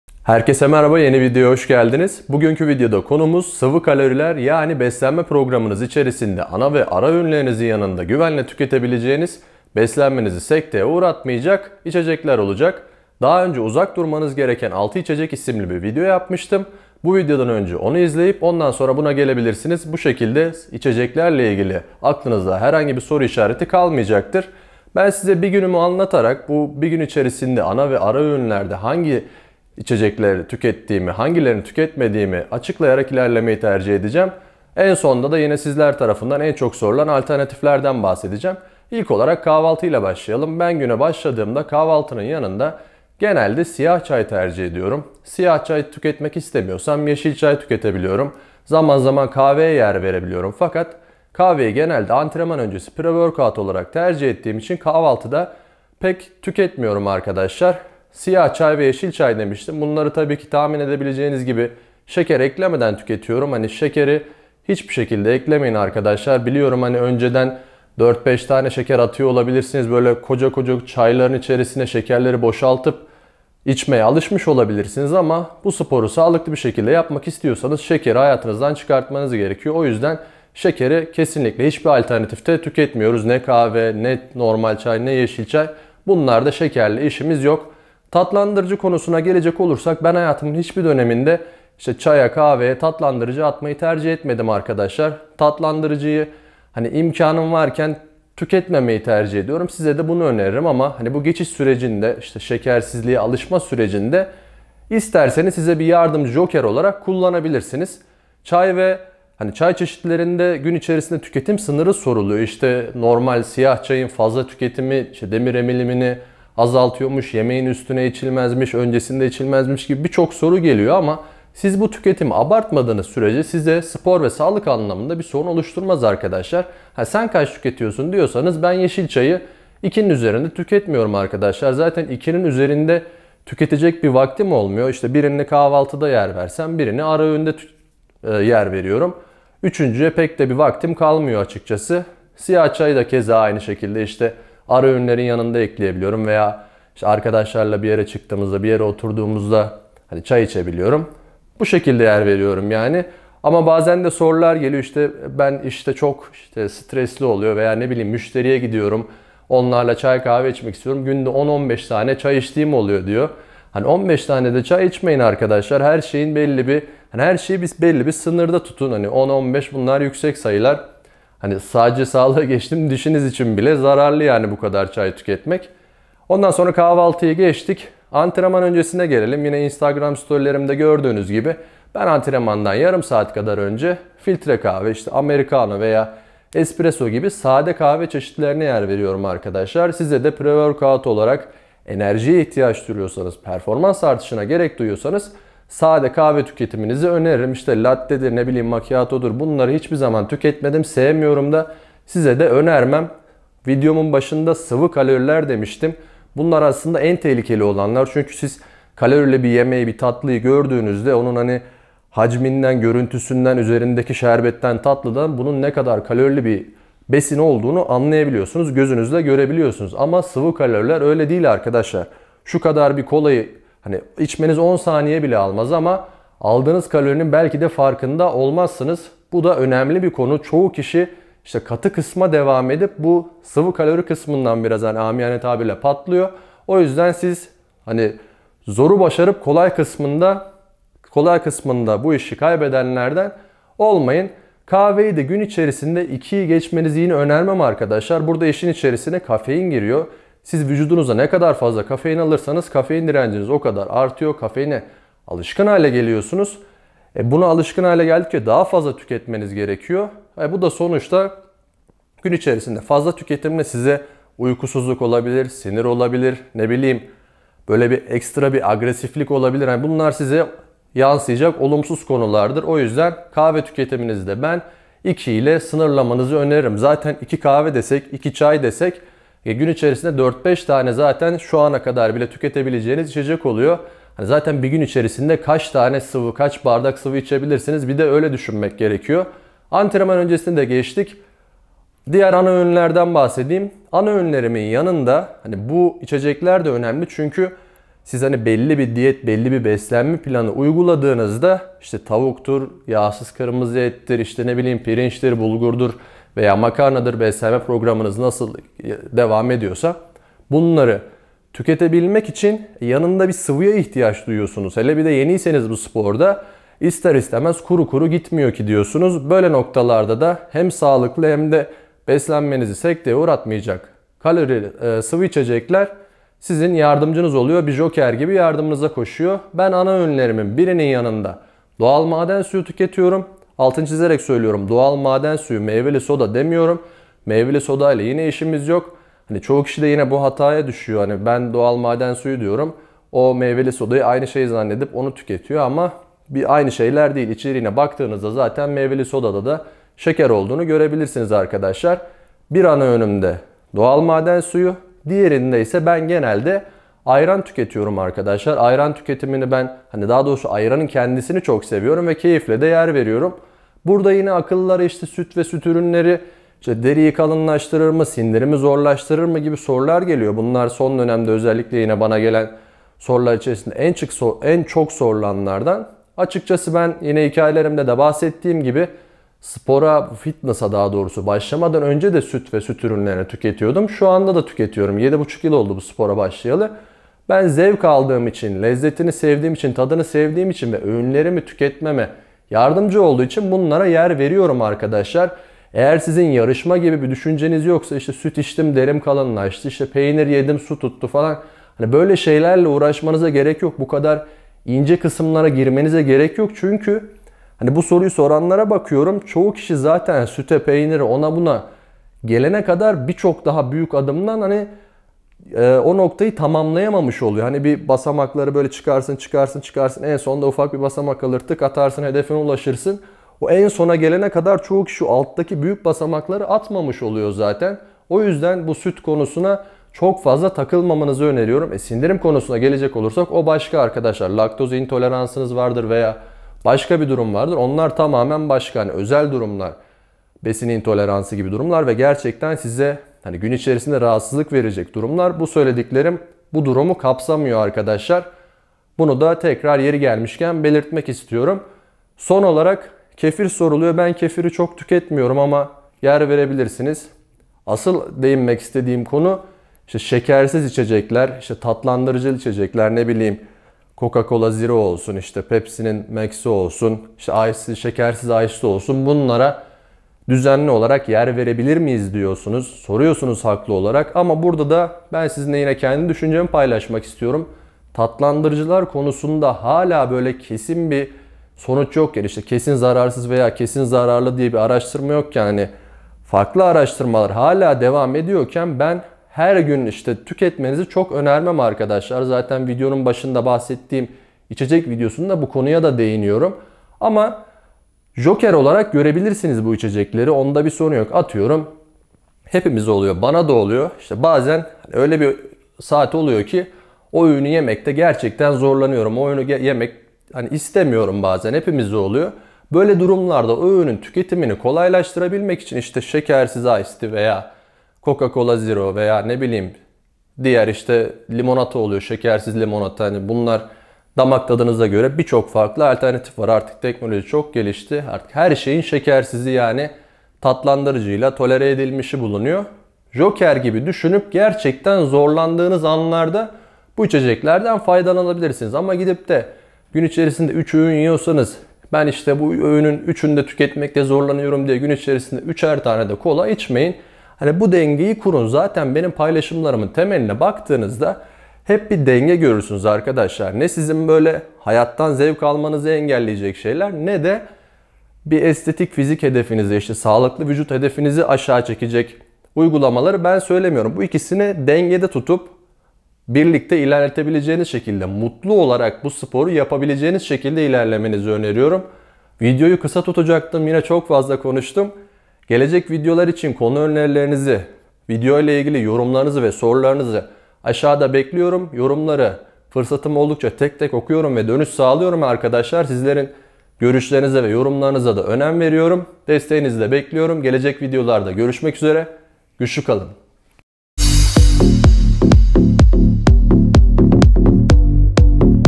MASUM İÇECEKLER | BESLENMENİZDE YER VEREBİLECEĞİNİZ SIVI KALORİLER
Masum içecekler! Beslenme programınızı aksatmayacak, ana ve ara öğünlerinizi tüketmenizi kolaylaştıracak sağlıklı içecek alternatiflerini kendi beslenme düzenimden örneklerle anlattım. Bu videoyu izledikten sonra sıvı kalorilerin kurbanı olmayacaksınız.

Fitness ve beslenmeyi sağlık & bilimi ön planda tutarak mühendis yaklaşımıyla ele aldığım kanalımda örnek antrenman programları, beslenme & antrenman tüyoları, fit tarifler ve samimi videolar bulabilirsiniz.